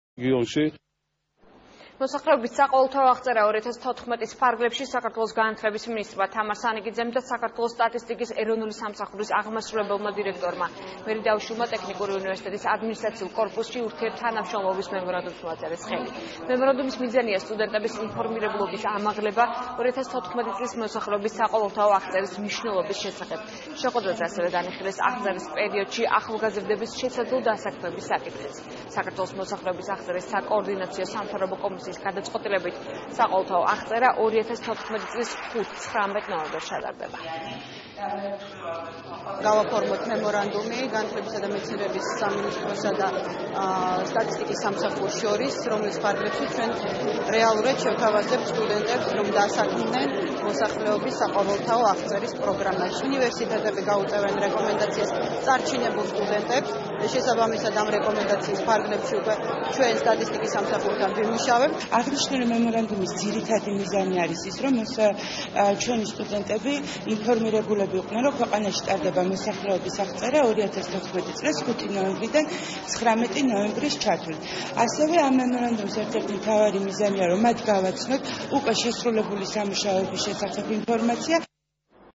საქართველოს განათლების სამინისტროსა და სტატისტიკის ეროვნულ სამსახურს შორის ურთიერთთანამშრომლობის მემ
11 ივნისს, მოსახლეობის საყოველთაო აღწერა 2014-ის ფარგლებში, საქართველოს განათლების მინისტრმა ქალბატონმა თამარ სანიკიძემ   და  საქართველოს სტატისტიკის ეროვნული სამსახურის აღმასრულებელმა დირექტორმა ქალბატონმა მერი დაუშვილმა ტექნიკური უნივერსიტეტის   ადმინისტრაციულ კორპუსში  ურთიერთთანამშრომლობის მემორანდუმს მოაწერეს ხელი. შეხვედრას საქართველოს ტექნიკური უნივერსიტეტის რექტორი ბატონი არჩილ ფრანგიშვილი და 200-მდე სტუდენტი დაესწრო. მემორანდუმის მიზანია სტუდენტების ინფორმირებულობის ამაღლება 2014 წლის მოსახლეობის საყოველთაო აღწერის მნიშვნელობის შესახებ.   შეხვედრაზე ასევე განიხილეს აღწერის პერიოდში ახალგაზრდების შესაძლო დასაქმების საკითხი.
 საქართველოს მოსახლეობის აღწერის საკოორდინაციო სამთავრობო კომისიის გადაწყვეტილებით მოსახლეობის საყოველთაო აღწერა 2014 წლის 5-19 ნოემბერს ჩატარდება.